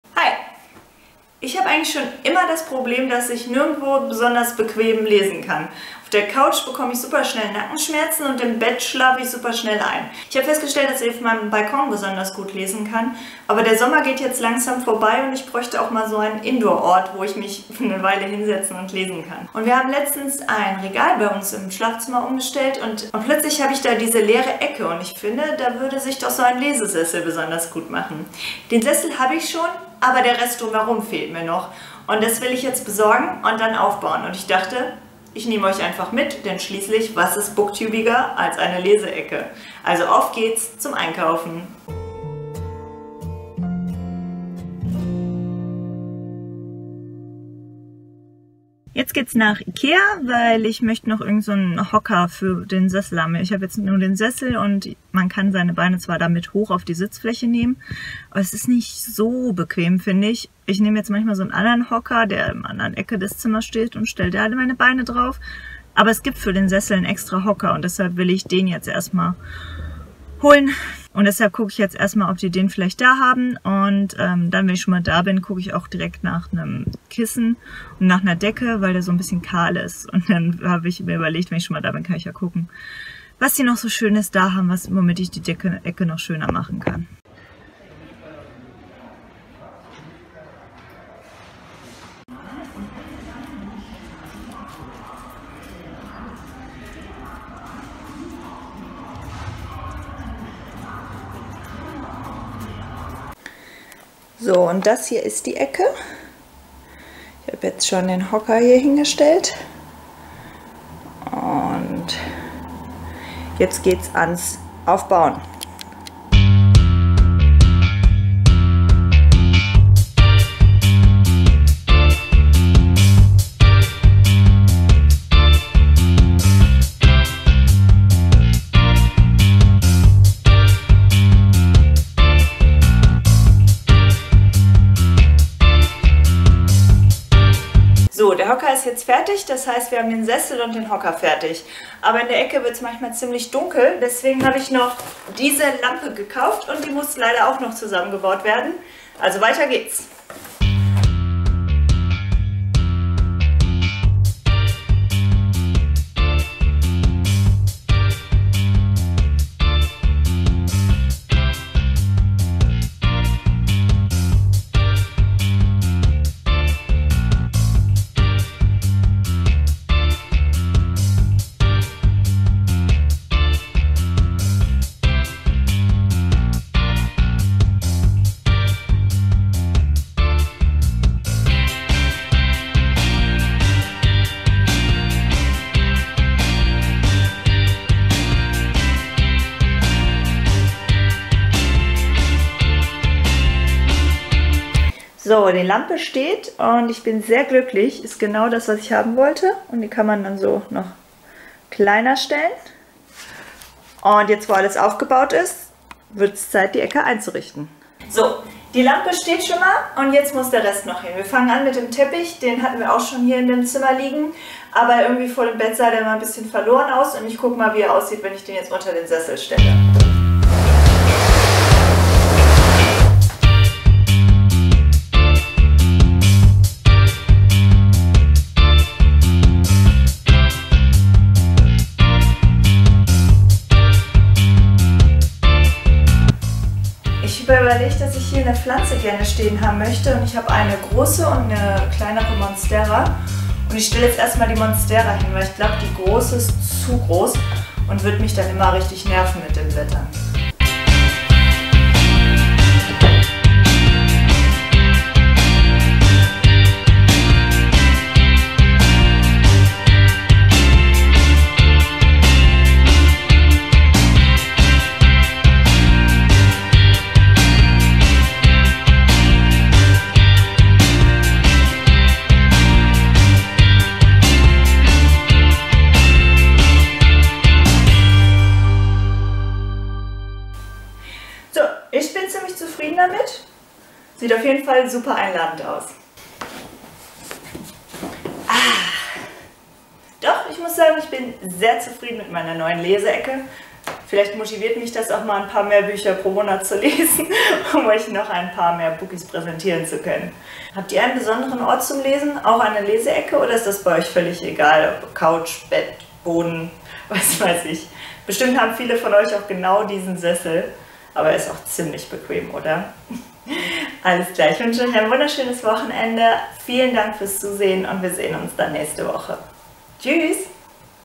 Meine neue Leseecke! | Lebst du noch oder liest du schon? | Aufbauvlog
[Unbezahlte Werbung]
Egal ob Bett, Couch, diverse Stühle - in meiner Wohnung gibt es einfach keinen Ort, an dem ich längere Zeit bequem lesen kann. Das werde ich jetzt ändern! Ich richte mir endlich eine Leseecke ein, damit der SuB in Angriff genommen werden kann 💪